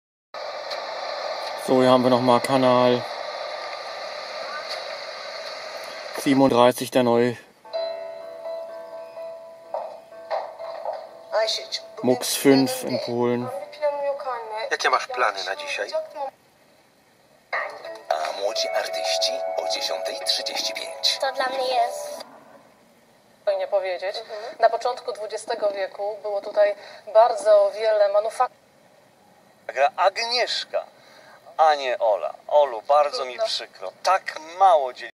01.06.2021, PCH1: Der neue TVP Test Mux-5, Szczecin K37 (2)
Hier nochmal der "TVP Test Mux-5", Szczecin-Kolowo, K37 um 07.54 MESZ mit starkem Signal und 100% Qualität

Here again "TVP Test Mux-5" from Szczecin-Kolowo, ch37  at 05:54 UTC. Now coming with strong signal and 100% of quality.

Content of "TVP Test DVB-T2 Mux-5"
TVP Dokument  (documentary px)
TVP Kobieta  (px for women; "Kobieta" means woman/female)
TVP Kultura HD (culture and religion)
TVP Polonia HD (external service for Polish speaking people)
TVP Rozrywka SD (entertainment px)